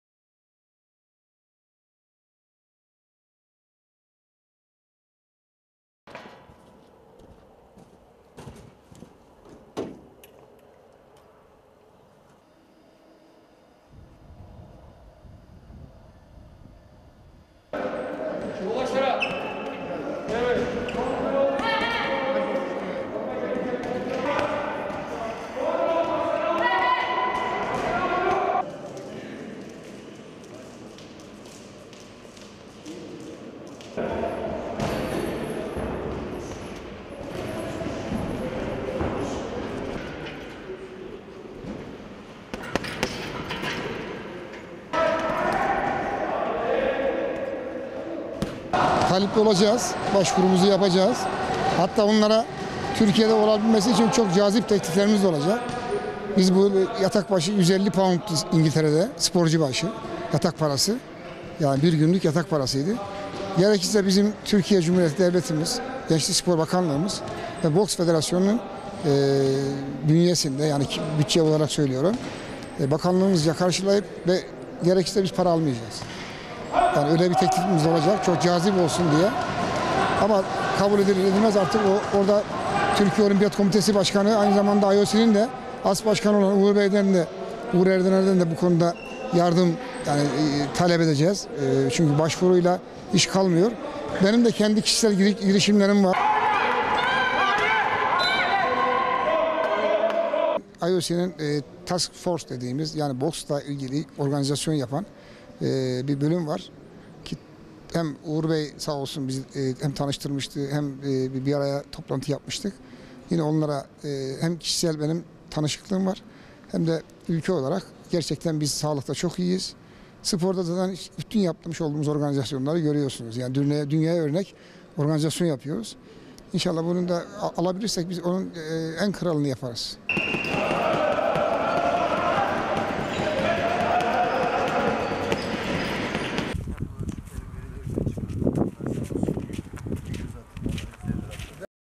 Türkiye, Tokyo Olimpiyatları Avrupa Kota Müsabakaları'na talip
GÖRÜNTÜ DÖKÜMÜ :
- Türkiye Boks Federasyonunun Kastamonu'daki Kadıdağı Kamp Merkezi'nden görüntü
- Türkiye Boks Federasyonu Başkanı Eyüp Gözgeç'den görüntü
- Erkek ve kadın olimpik boks milli takımlarından görüntü
- Türkiye Boks Federasyonu Başkanı Eyüp Gözgeç ile röportaj Türkiye, Tokyo Olimpiyatları Avrupa Kota Müsabakaları'na talip
- Türkiye Boks Federasyonu Başkanı Eyüp Gözgeç:
- "Uluslararası Olimpiyat Komitesine (IOC) başvuruda bulunacağız. Türkiye'de yapılması için çok cazip tekliflerimiz olacak"
- "Dünyaya örnek organizasyon yapıyoruz. Bunu alabilirsek en kralını yaparız"
KASTAMONU (AA) - BİLAL KAHYAOĞLU - Türkiye Boks Federasyonu Başkanı Eyüp Gözgeç, Tokyo Olimpiyatları için Londra'da nisan ayında yapılması planlanan Avrupa Kota Müsabakaları'nın iptal edilmesinin ardından bu organizasyona talip olduklarını söyledi.
Gözgeç, AA muhabirine, İngiltere'de artan yeni tip koronavirüs (Kovid-19) vakaları nedeniyle organizasyonun Londra'da yapılmasından vazgeçildiğini belirtti.
Bu sürecin ardından organizasyonun Türkiye'de yapılması için çalışmalara başladıklarını anlatan Gözgeç, "Avrupa Kota Müsabakaları'na talip olacağız. Uluslararası Olimpiyat Komitesine (IOC) başvuruda bulunacağız. Türkiye'de yapılması için çok cazip tekliflerimiz olacak. İngiltere'de sporcu başı bir günlük yatak parası 150 pound. Türkiye Cumhuriyeti Devleti, Gençlik ve Spor Bakanlığımız, Boks Federasyonu bünyesinden karşılayıp gerekirse para almayacağız. Cazip olsun diye böyle bir teklifimiz olacak. Türkiye Milli Olimpiyat Komitesi Başkanı Uğur Erdener'den de yardım talep edeceğiz. Benim de kendi kişisel girişimlerim var." ifadelerini kullandı.
Gözgeç, organizasyonunun Türkiye'de yapılmasının Türk boksörlere avantaj sağlayacağını dile getirerek, "Psikolojik olarak sporcular üzerinde çok büyük etkisi olacak. Türkiye'nin dışındaki her yer bize gurbet ama kendi evimizde olunca sporculara öz güven gelecek. Organizasyonda maske, mesafe ve hijyen kurallarına uyacağız. Bir otelin içinde 7 günlük yaşam olacak. Ondan sonra da sağ salim memleketlerine göndereceğiz. Tarih değişmezse nisan ayının 22'sinde olacak." diye konuştu.
Kovid-19 sürecini Türkiye'nin Avrupa ülkelerine kıyasla çok daha iyi yönettiğini vurgulayan Gözgeç, "Ülke olarak yaptığımız organizasyonları görüyorsunuz. Dünyaya örnek organizasyon yapıyoruz. Bunu alabilirsek en kralını yaparız." değerlendirmesinde bulundu.